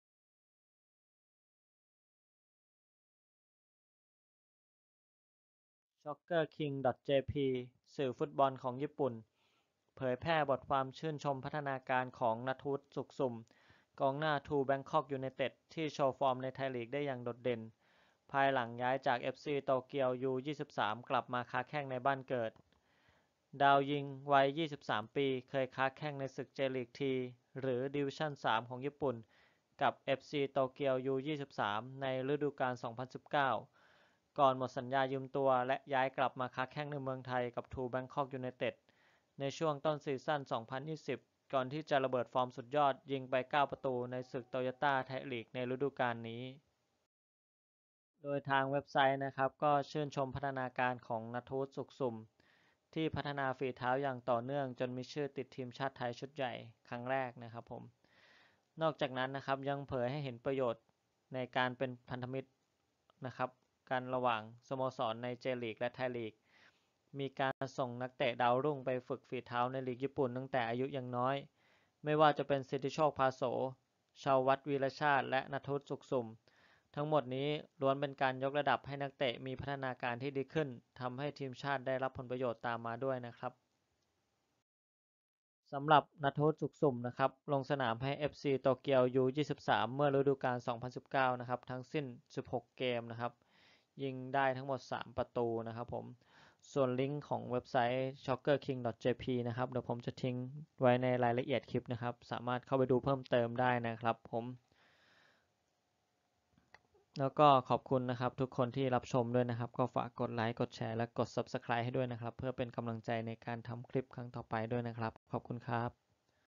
สื่อชื่อดังของญี่ปุ่นชื่นชมพัฒนาการของ ณัฐวุฒิ สุขสุ่ม หลังโชว์ฟอร์มกับต้นสังกัดได้อย่างยอดเยี่ยม
Soccerking.jp สื่อฟุตบอลของญี่ปุ่น เผยแพร่บทความชื่นชมพัฒนาการของ ณัฐวุฒิ สุขสุ่ม กองหน้าทรู แบงค็อก ยูไนเต็ด ที่โชว์ฟอร์มในไทยลีกได้อย่างโดดเด่น ภายหลังย้ายจากเอฟซี โตเกียว U23 กลับมาค้าแข้งในบ้านเกิด
ดาวยิงวัย 23 ปี เคยค้าแข้งในศึกเจลีก 3 ญี่ปุ่น กับ เอฟซี โตเกียว U23 ในฤดูกาล 2019 ก่อนหมดสัญญายืมตัวและย้ายกลับมาค้าแข้งในเมืองไทยกับ ทรู แบงค็อก ยูไนเต็ด ในช่วงต้นซีซั่น 2020 ก่อนที่จะระเบิดฟอร์มสุดยอดยิงไป 9 ประตูในศึกโตโยต้า ไทยลีก ฤดูกาลนี้
โดยเว็บไซต์ Soccerking.jp ตีแผ่บทความชื่นชมพัฒนาการของ ณัฐวุฒิ สุขสุ่ม ที่ได้มาจากการย้ายไปเล่นในเจลีก 3 และกลับมาพัฒนาฝีเท้าอย่างต่อเนื่องจนมีชื่อติดทีมชาติไทยชุดใหญ่ครั้งแรก
นอกจากนั้นยังเผยให้เห็นถึงประโยชน์ในการเป็นพันธมิตรร่วมกันระหว่างสโมสรในเจลีกและไทยลีก มีการส่งนักเตะดาวรุ่งไปฝึกฝีเท้าในลีกญี่ปุ่นตั้งแต่อายุยังน้อย ไม่ว่าจะเป็น สิทธิโชค ภาโส, เชาว์วัตน์ วีระชาติ และ ณัฐวุฒิ สุขสุ่ม ทั้งหมดนี้ล้วนเป็นการยกระดับให้นักเตะมีพัฒนาการที่ดีขึ้น ทำให้ทีมชาติได้รับผลประโยชน์ตามมาด้วย
สำหรับ ณัฐวุฒิ สุขสุ่ม ลงสนามให้ เอฟซี โตเกียว U23 เมื่อฤดูกาล 2019 ทั้งสิ้น 16 เกม ยิงไป 3 ประตู